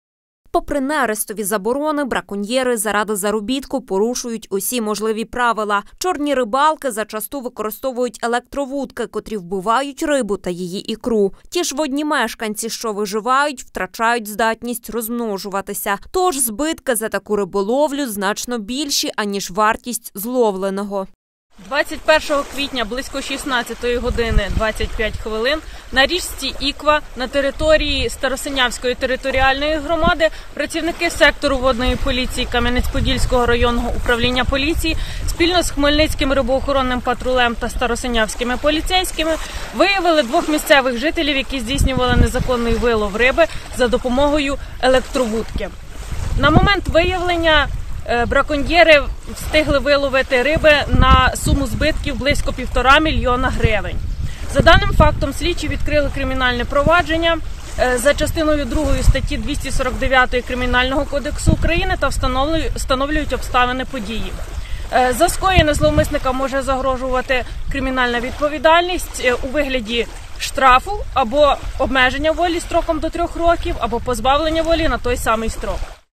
Хмельниччина: браконьєри «нарибалили» на понад півтора мільйони гривень
На Хмельниччині браконьєри ловили рибу за допомогою електоровудок, чим завдали шкоди на величезну суму. Наразі в області триває операція «Нерест», тож такі дії не лише порушують закон, а й ставить під загрозу природне відтворення іхтіофауни. Де рибалили браконьєри та яке покарання можуть понести – розповіли у Кам’янець-Подільському районному управлінні поліції.